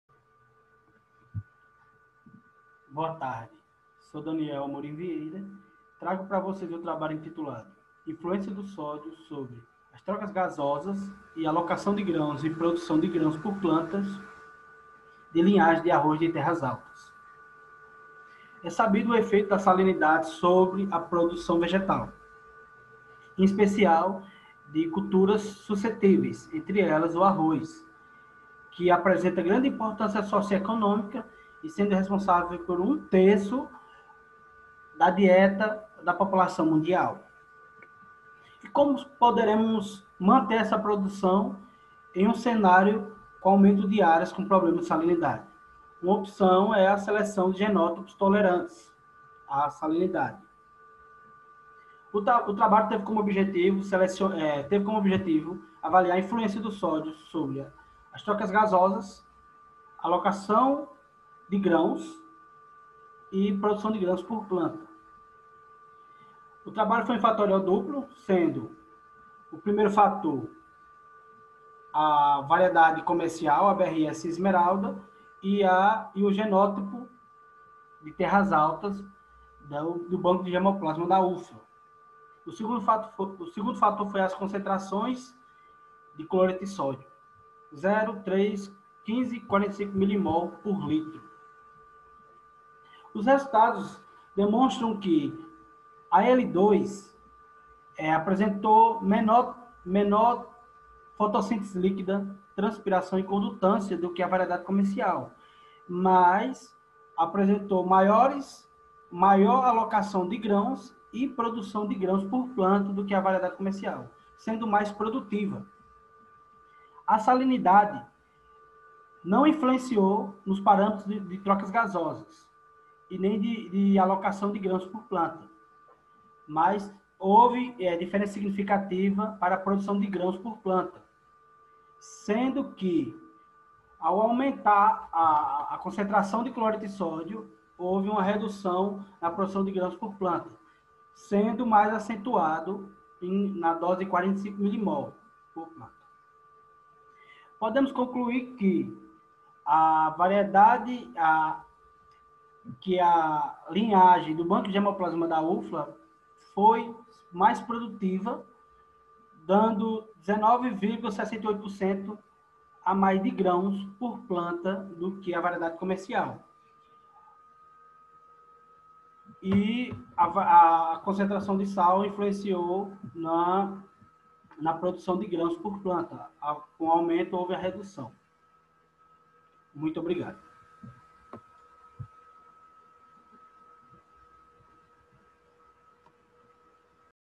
Influence Of Sodium On Gas Exchange And Allocation Of Biomass In High Land Rice Lines
Vieira, D.A¹, Toro Herrera, M.A¹, Pennacchi, J.P¹, Botelho, F.B.S2, Barbosa, J.P.R.A.D¹
Rice is of great socioeconomic importance as it is part of the diet of one third of the world population. Increases in rice production will be required, mainly considering the expected sharp increase in global population and the predicted future scenarios of climate change and limited expansion of agricultural land. In this scope, rice cultivation may be directed to areas with salty soils. The aim of the present study was to evaluate the effects of sodium chloride (NaCl +) doses on the physiological aspects and on the biomass allocation of two upland and short-cycle lines of the Federal University of Lavras (UFLA) rice germplasm bank. The experiment was carried out in a greenhouse at UFLA with rice seeds sowed in Citropote containers with a volume of 4 liters and substrate in the composition 1: 2 (sandy:clay textures). The experiment was designed in completely randomized blocks, with 5 replicates and in a factorial scheme, with the two factor being: 2 rice lines, L1 (BRS ESMERALDA) and L2 (CMG 2085), and the 4 NaCl + concentrations (0; 3; 15; 45 mmol L-1), 5 repetitions, totaling 40 plots. The evaluated characteristics were: liquid photosynthesis, transpiration, stomatal conduction, biomass allocation and grain production per plant. There was no interaction between the factors. The results obtained indicated that L2 was more efficient in the allocation of photoassimilates for filling the grains, obtaining a higher allocation and productivity per plant of 8.668 g compared to 6.962 g of L1, even with lower values in gas exchange. For the salt treatments, the concentration of 3 mmol L-1 of NaCl + was shown to be similar to the control treatment in terms of gas exchange, being higher in terms of allocation of photoassimilates and grain yield. Above this concentration there was a reduction in all variables evaluated. The higher salt content reduced all the variables evaluated. L2 was more promising, as it produced 19.68% more grains per plant than L1.

Acknowledgments: CNPq, CAPES, UFLA